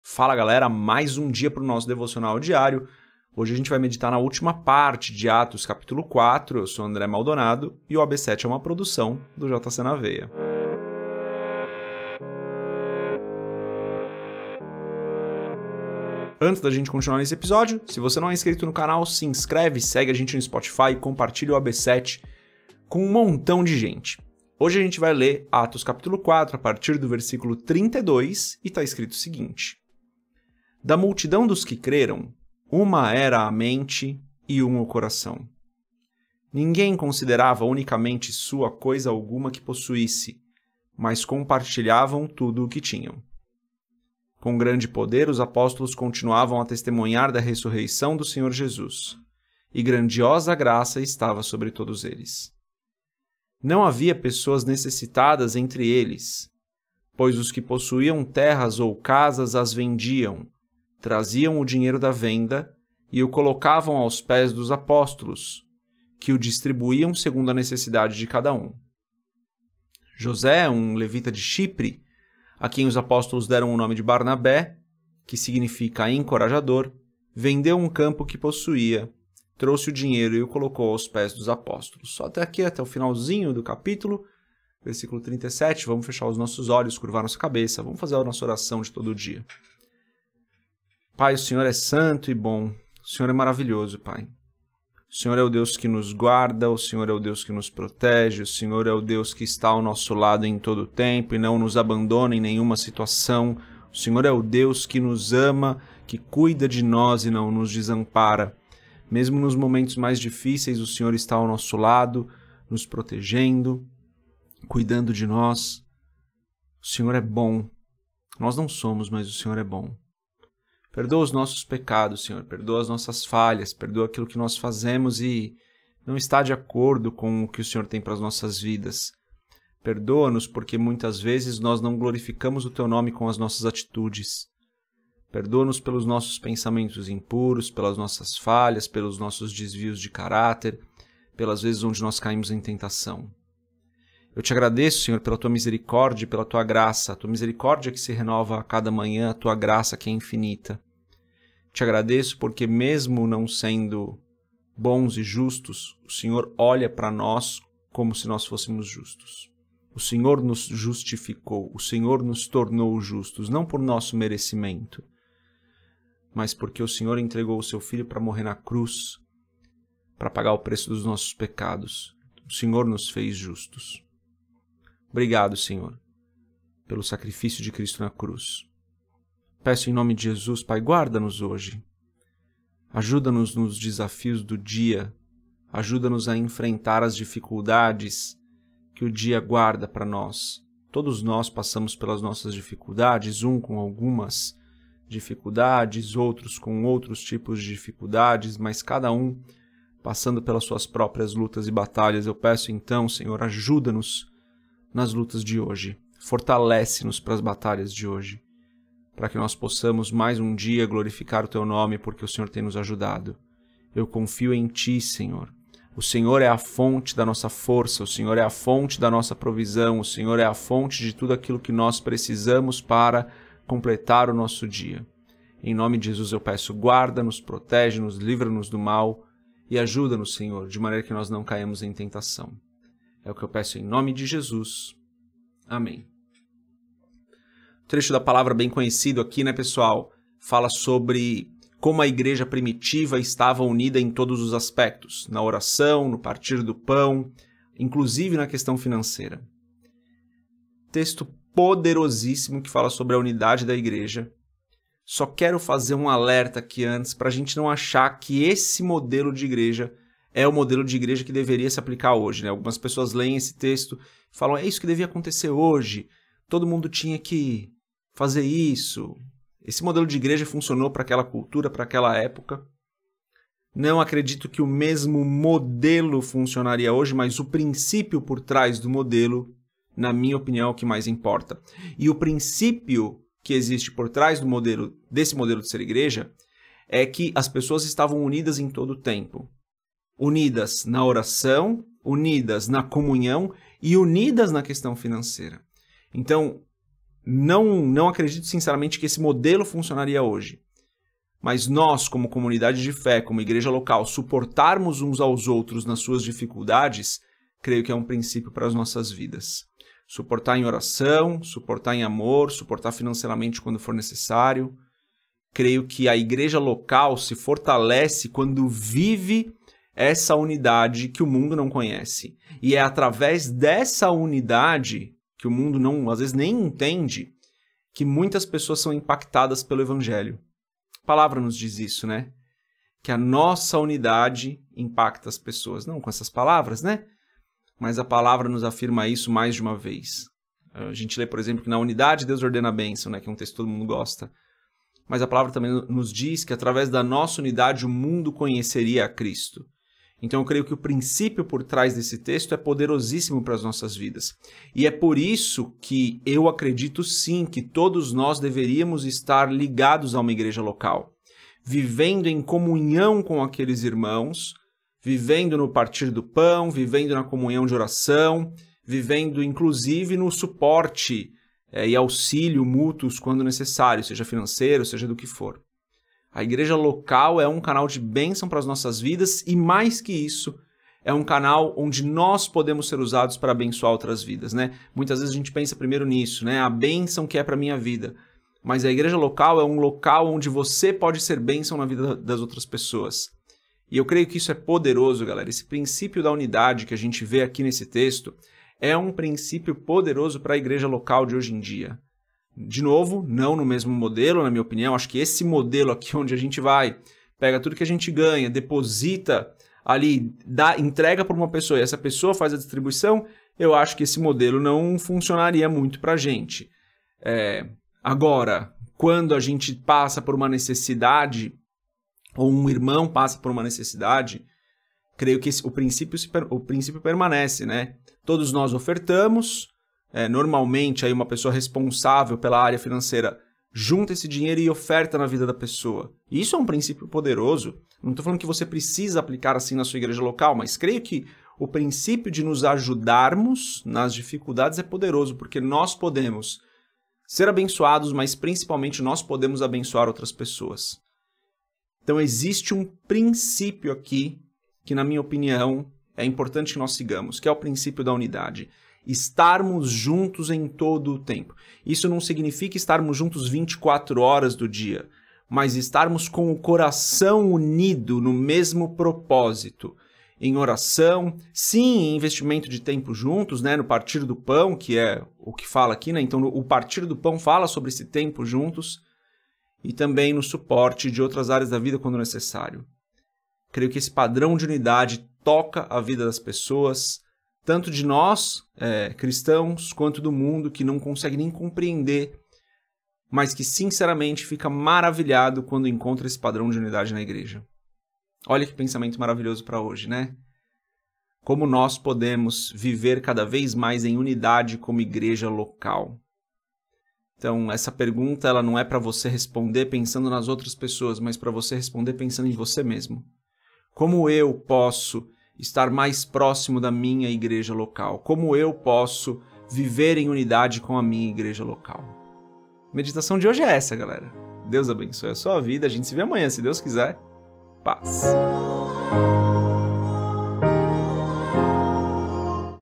AB7 - Devocional diário - Atos 4 - Parte 5
Esse é o nosso devocional diário, o AB7. Hoje falamos sobre a quinta parte de Atos 4.